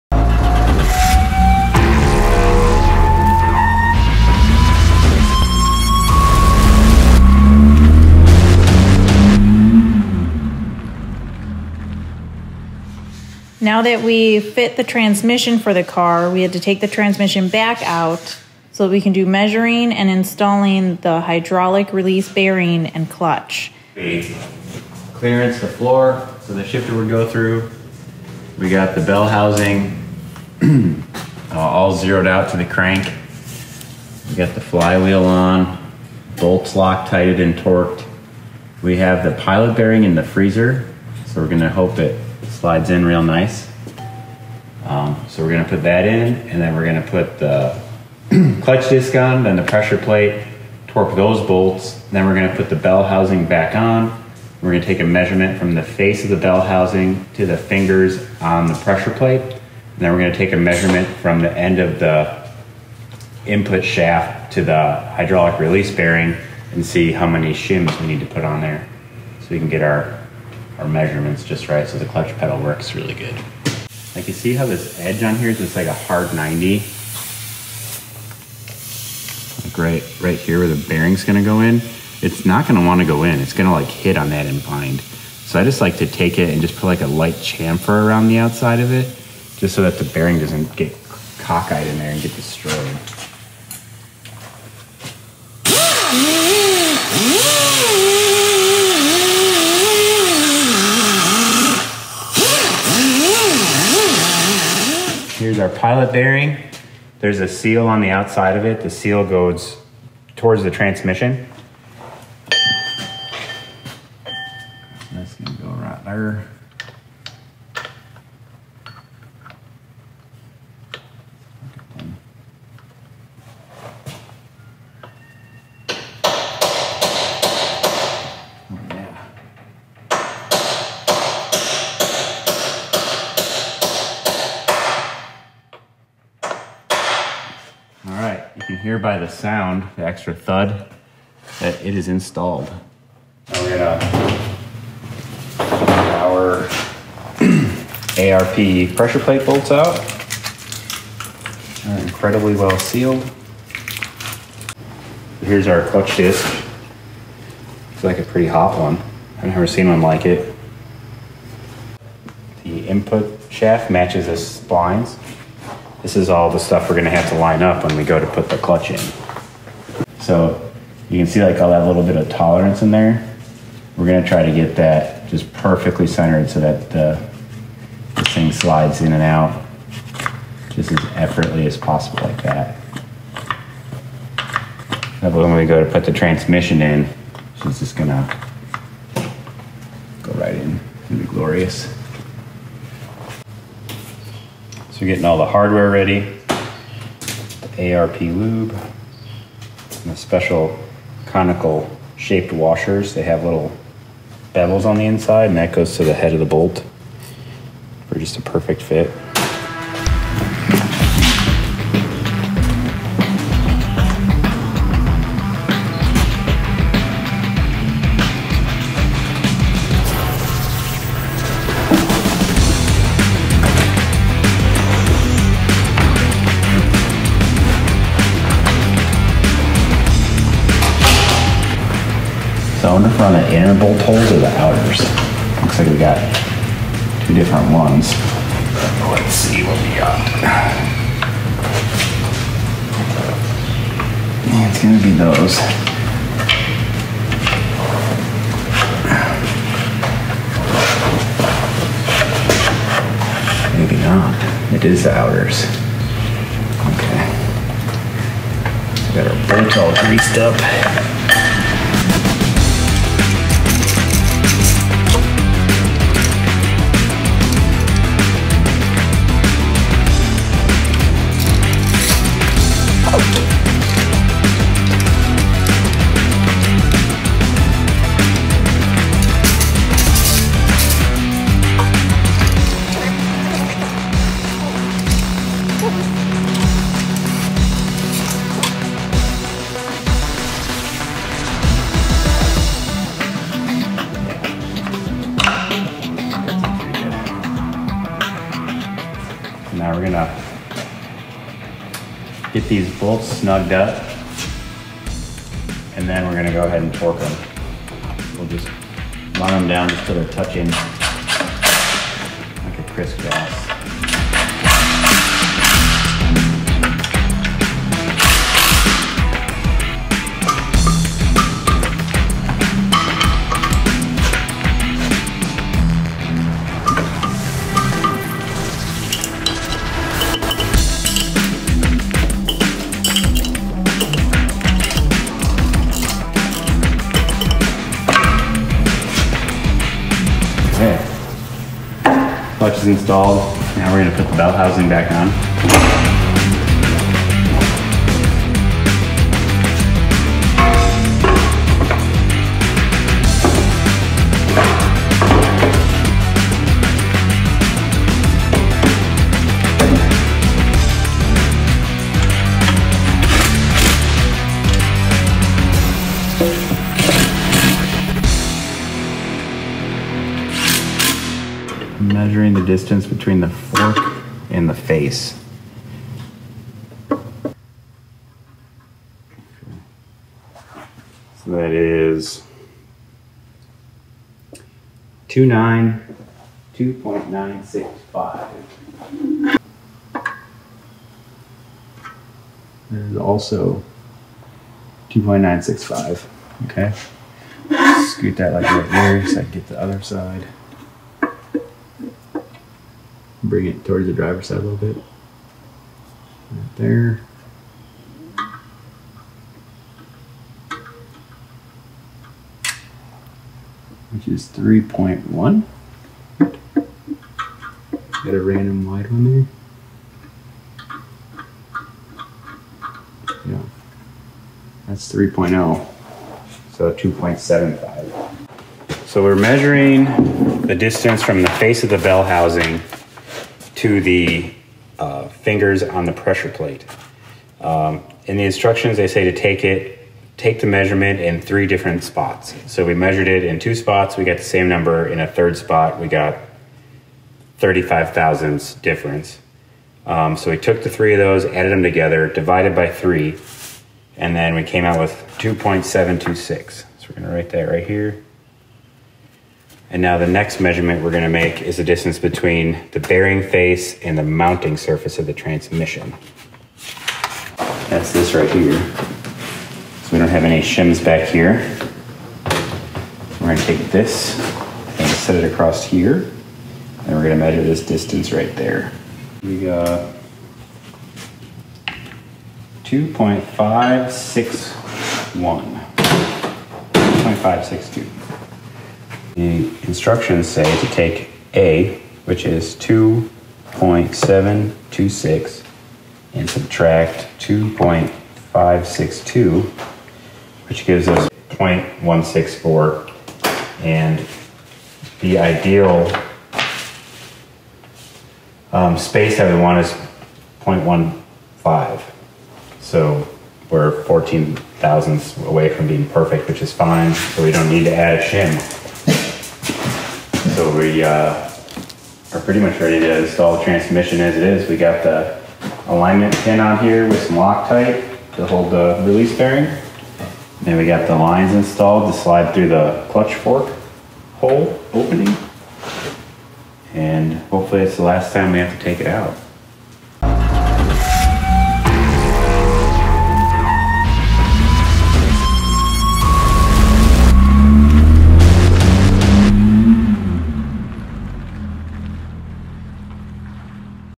Measuring & Installing Hydraulic Release Bearing & Clutch Tremec TKX 5 Speed Trans in 77 Corvette C3
Measuring & Installing Hydraulic Release Bearing & Clutch TKX 5 Speed Transmission for this 77 Corvette.  We are converting this 77 Corvette from an automatic to manual using the C3 Corvette TKX 5-Speed Kit from American PowerTrain.  Other upgrades planned include a new Vintage Air conditioning system and a new Holley Sniper fuel injection system.  Follow us as we show each major step along the way.